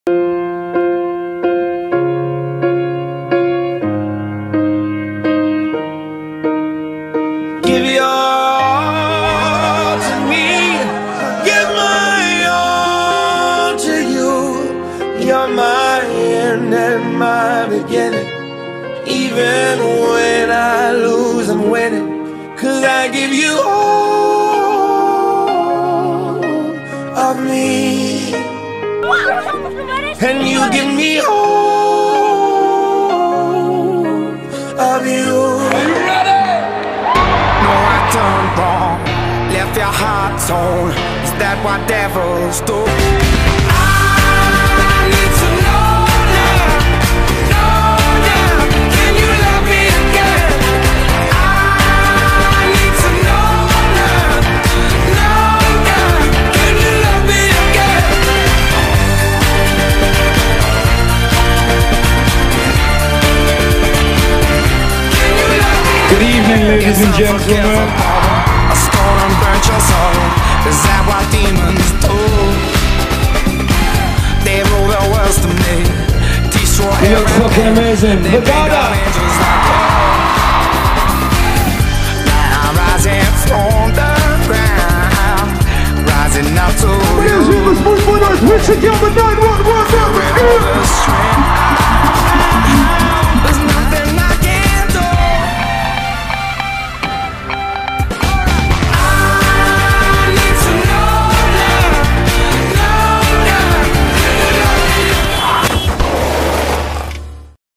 MEO Marés Vivas 2015
O maior festival a norte começou em grande ao som de Capicua, Blind Zero, John Newman, John Legend e Richie Campbell!